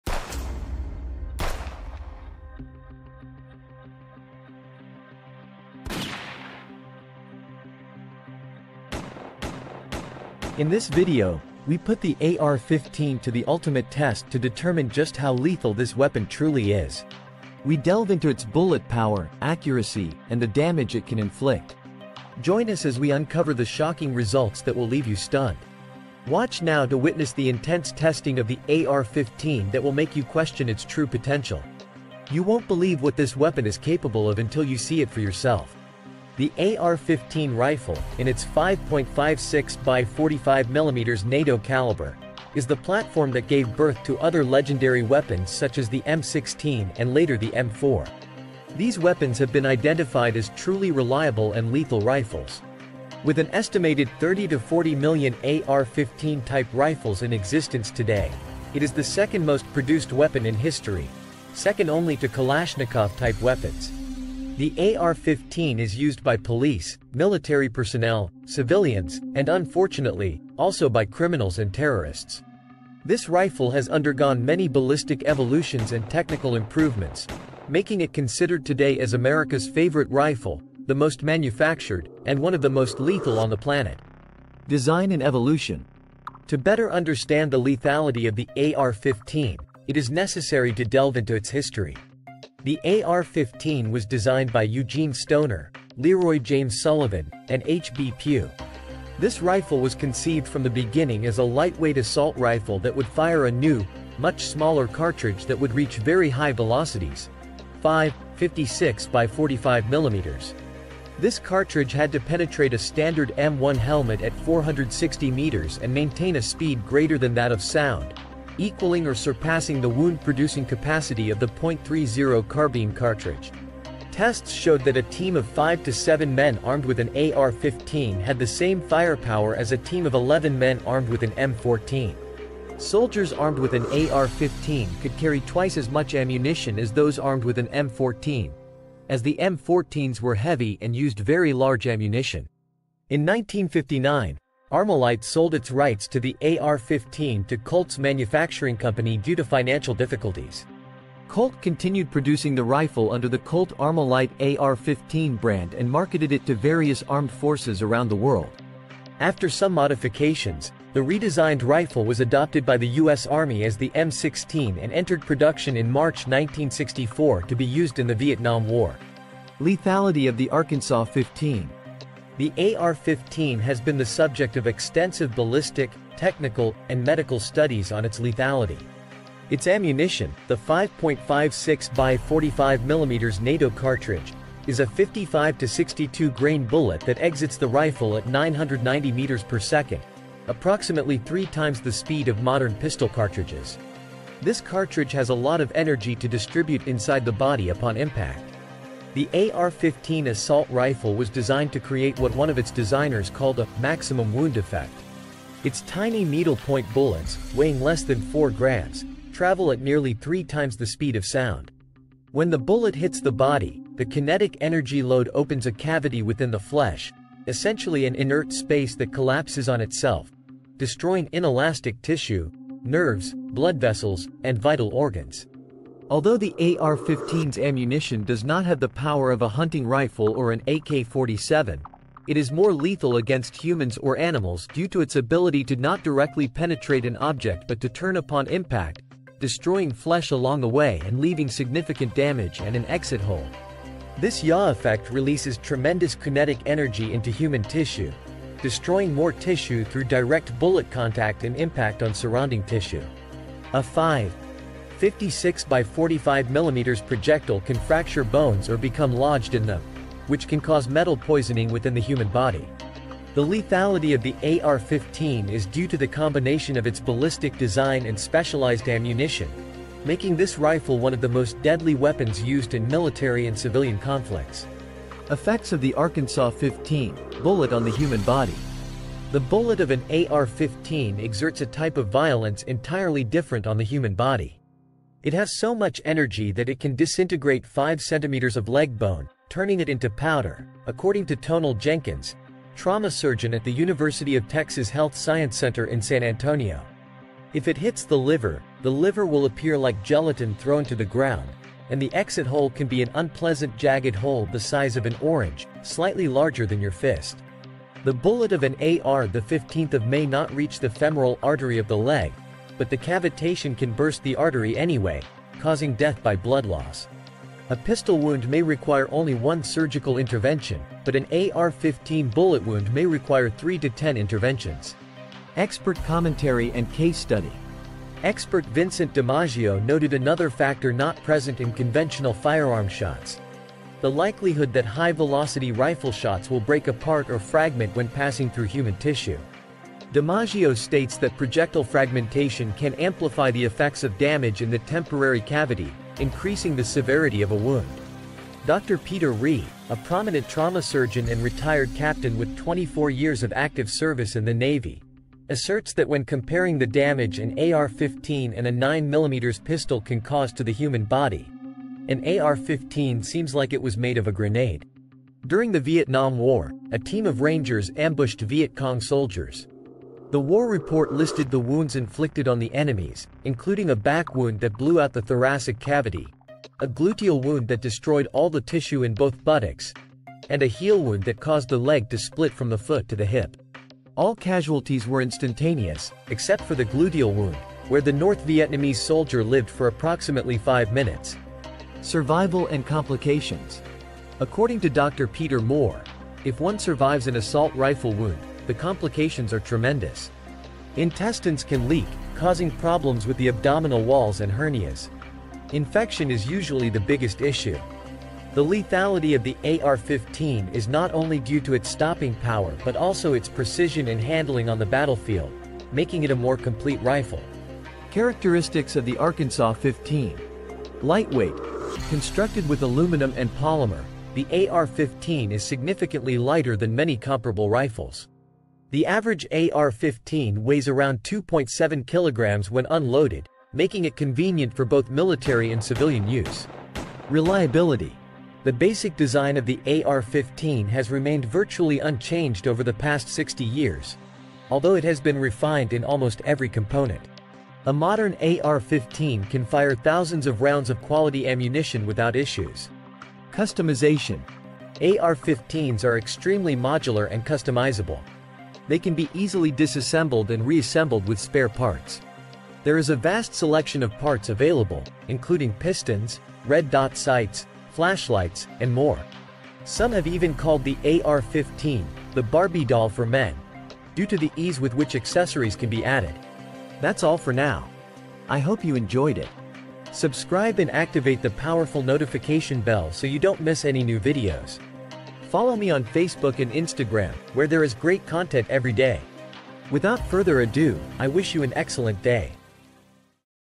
The Hidden Dangers of AR-15 Ownership
We put the AR-15 to the ultimate test to determine just how lethal this weapon truly is. We delve into its bullet power, accuracy, and the damage it can inflict. The AR15 rifle in its 5.56 x 45 mm NATO caliber is the platform that gave birth to other legendary weapons such as the M16 and later the M4, weapons that since time immemorial have been identified as truly reliable and lethal rifles.